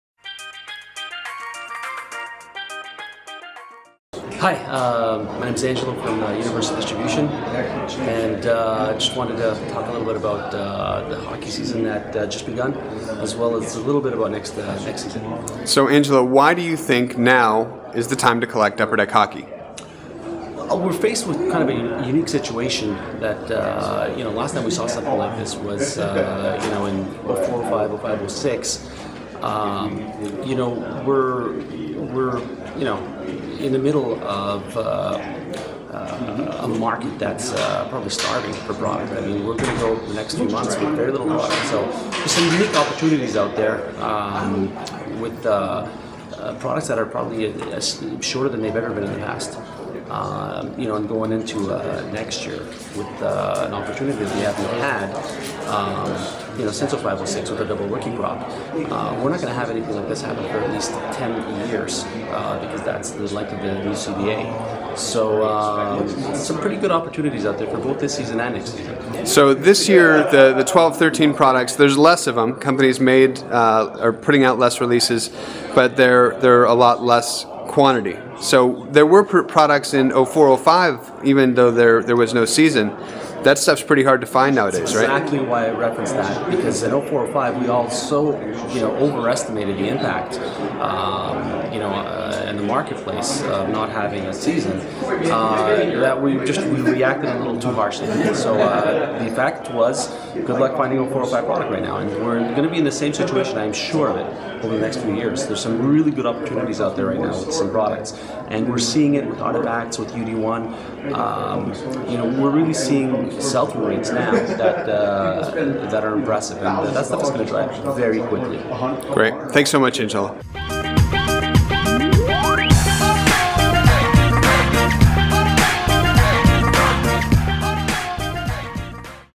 The 2013-14 NHL Double Rookie Class Industry Impact - Universal
Now is the time to collect Upper Deck Hockey. Listen as Angelo Exarhakos from Universal Distribution talks about the 12-13 NHL season and the double rookie class coming in 13-14. For more information on Upper Deck NHL products, please for more details.